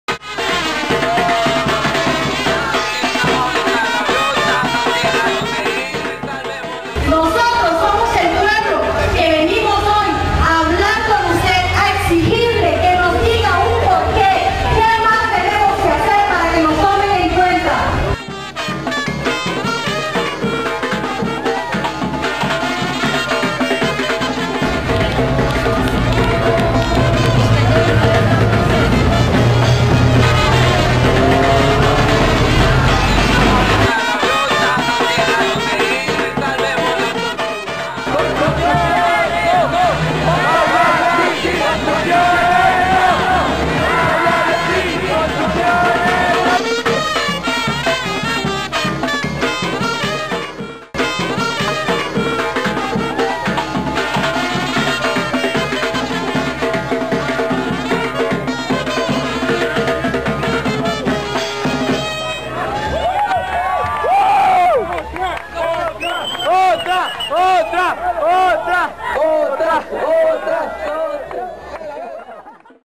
Leatherback sea turtle defends Las Baulas Marine Park
A Leatherback sea turtle protests at the Costarican Presidential house to secure Las Baulas Marine Park againts urban development.